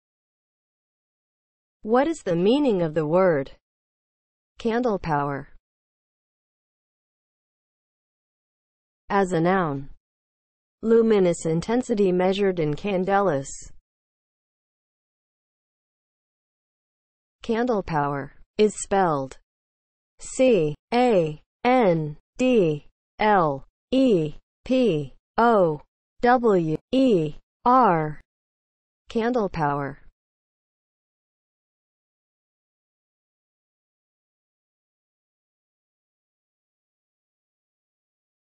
What is the meaning of the word CANDLEPOWER?
Definition and spelling of the word CANDLEPOWER.

(00:00) Intro
(00:08) As a noun
(00:16) Spelling

As a noun (00:08):
• luminous intensity measured in candelas